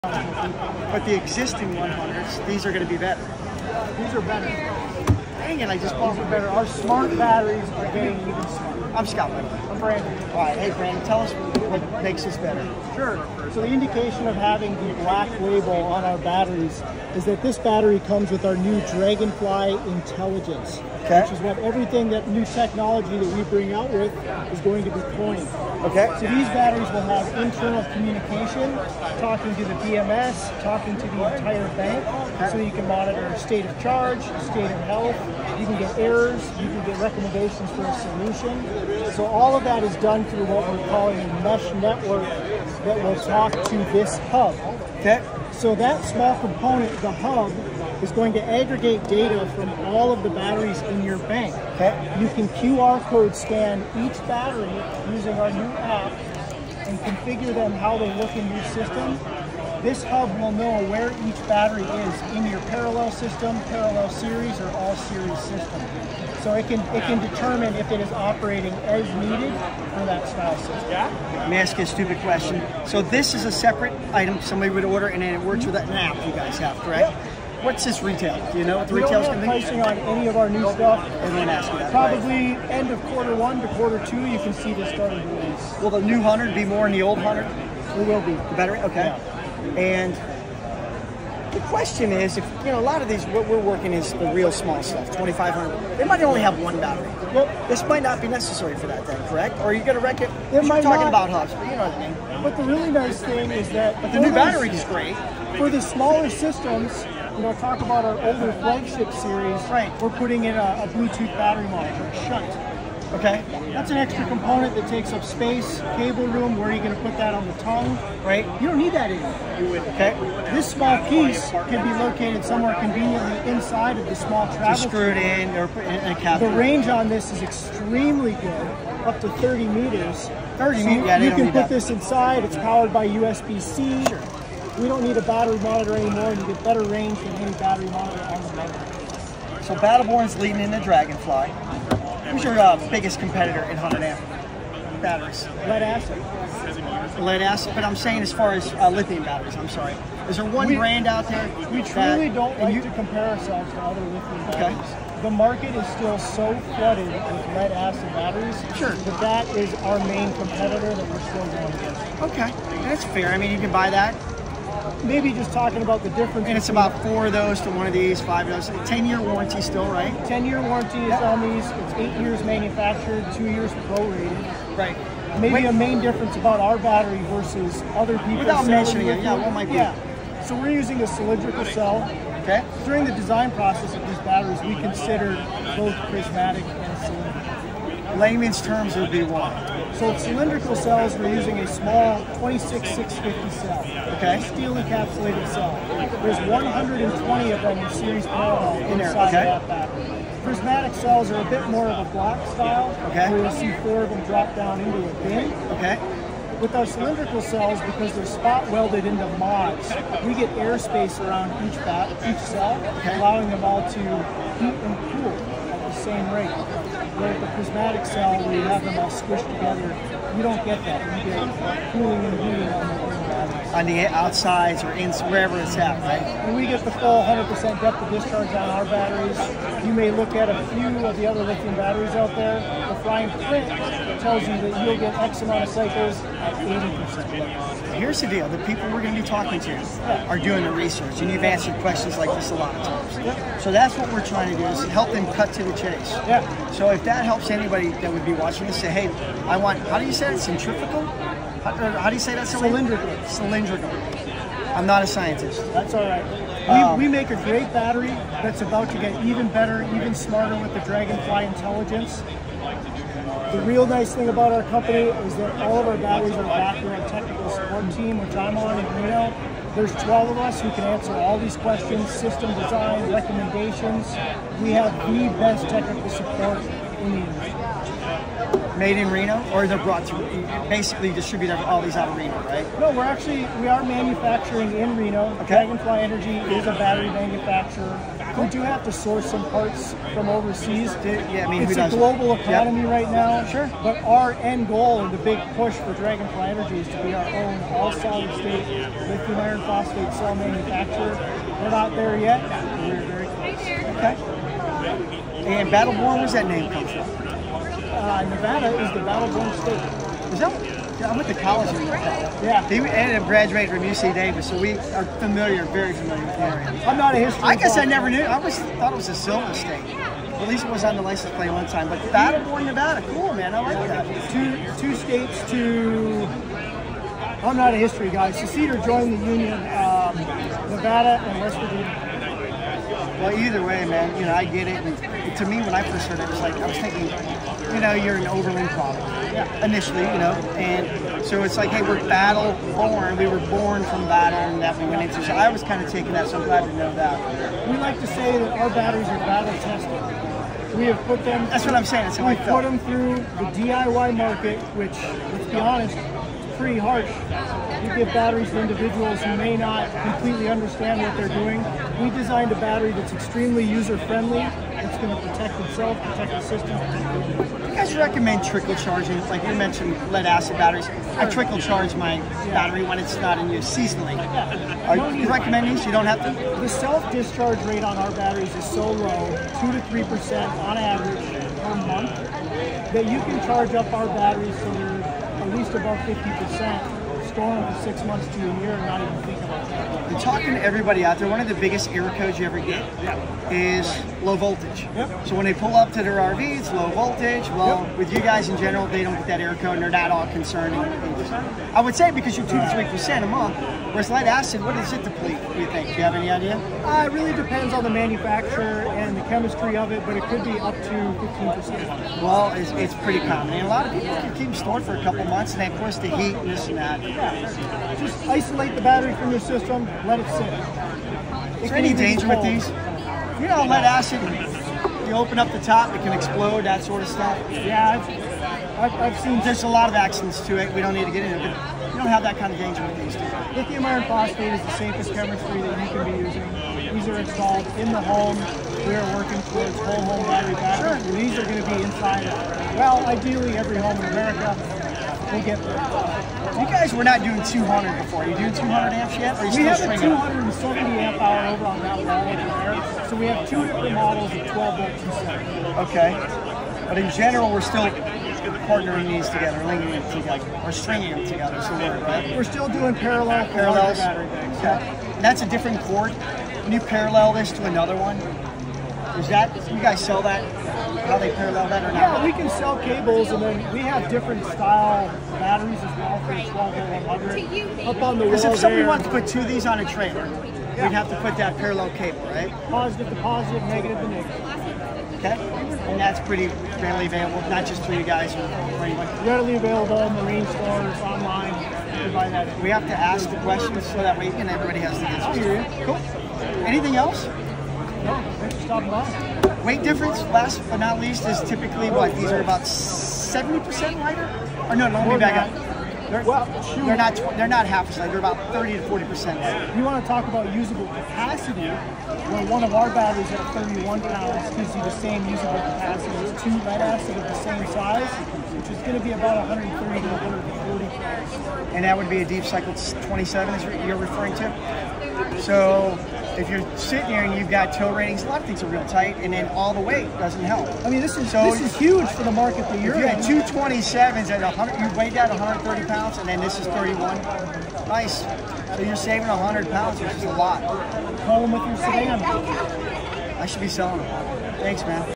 New Smart Lithium Battery by Battle Born
Join Scott as he goes through Battle Born’s Smart Tech Lithium Battery with one Battle Born’s own experts.

We are on the hunt to see which Lithium battery best suits our off the grid needs, so tag your favorite in the comments so we can check them out.

** apologies for video shot in vertical mode, but still thought the information was more than worth sharing.